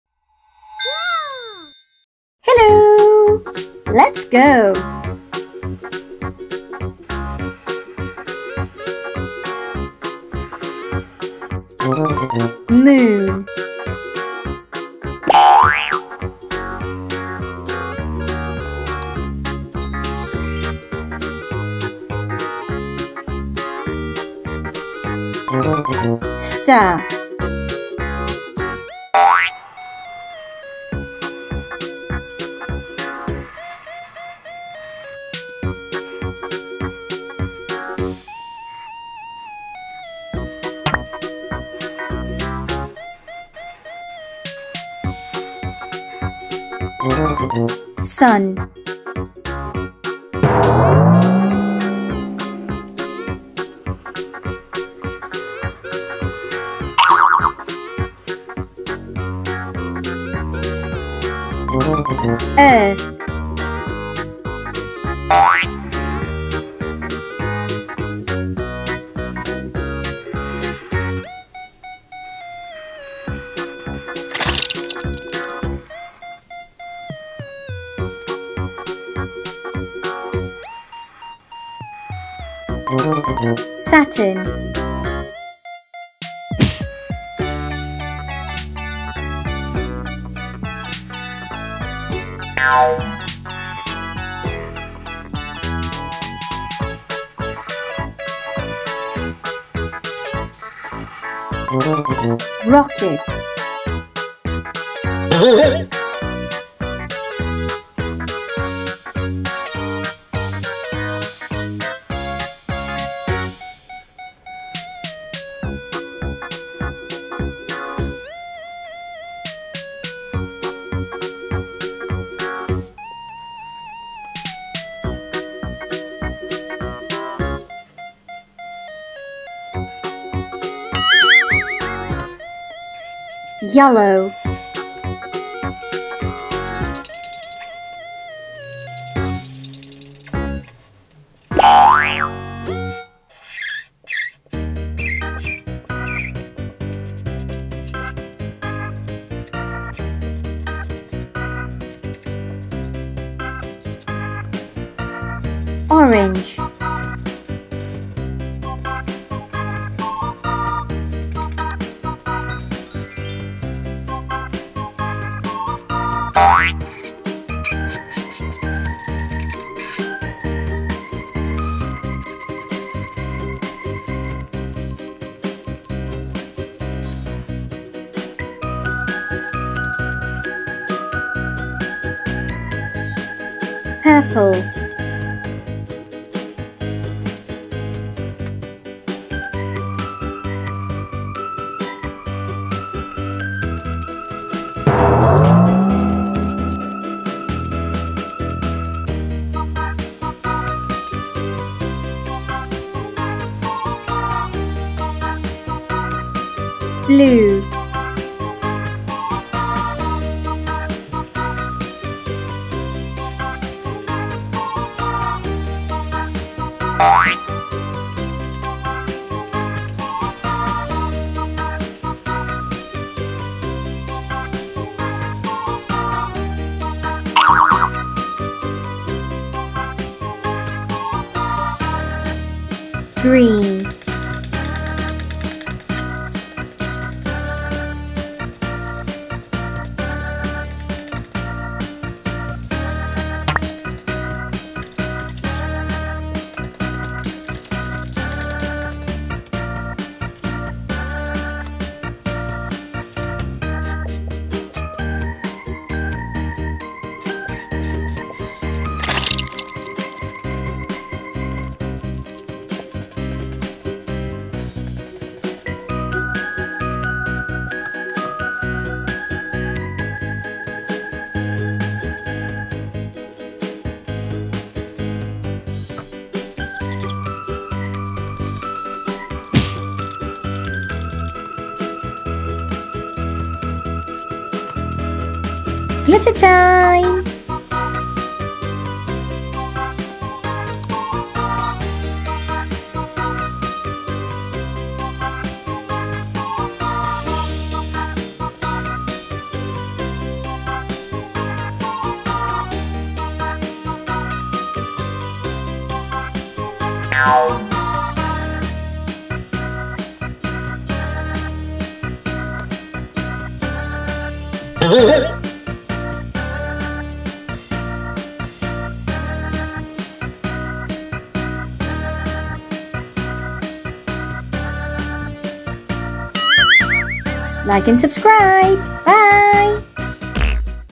Solar System Coloring and Drawing for Kids, Toddlers
Hello

Let's draw and color Our Solar System in this video.